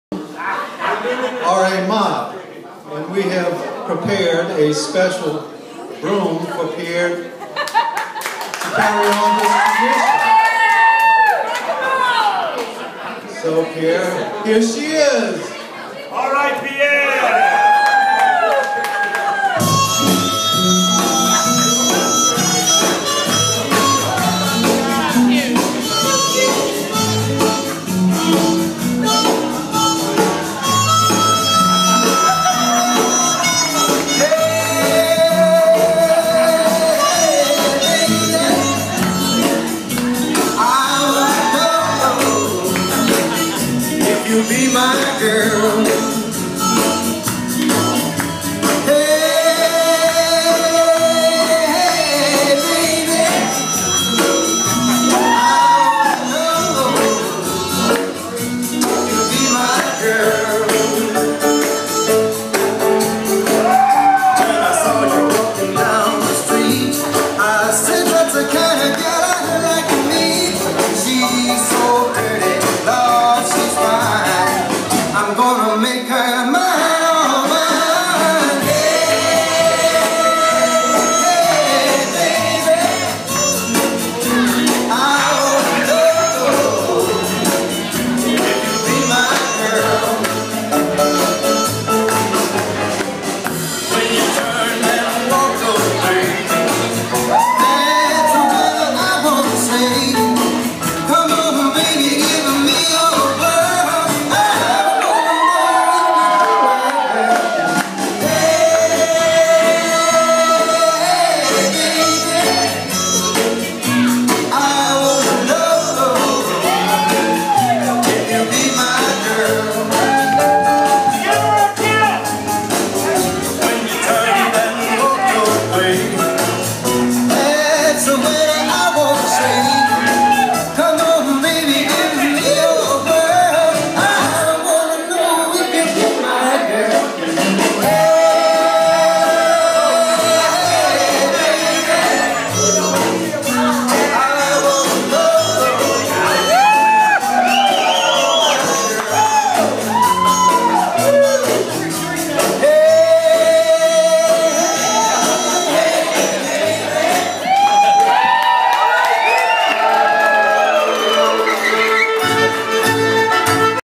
Wedding Cajun Custom , dancing with the broom
In wedding custom.  The oldest Un married male sibling of the bride must dance with a broom.  This is Pierre, Monique's  brother dancing at their sister Juliette's wedding.